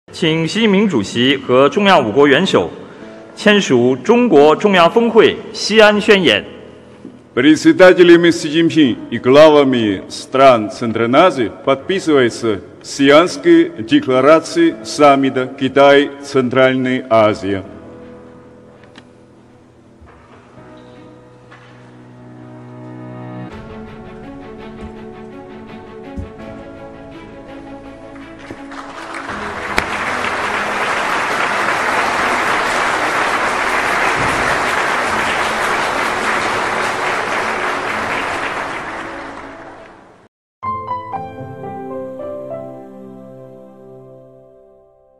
Xi attends signing ceremony with leaders of five Central Asian countries
Chinese President Xi Jinping on Friday attended a joint signing ceremony and met the press with leaders of five Central Asian countries at the China-Central Asia Summit held in the city of Xi'an, northwest China's Shaanxi Province.
The summit, which runs from Thursday to Friday, is the first in-person gathering among heads of state of China and five Central Asian countries since China established diplomatic ties with them 31 years ago.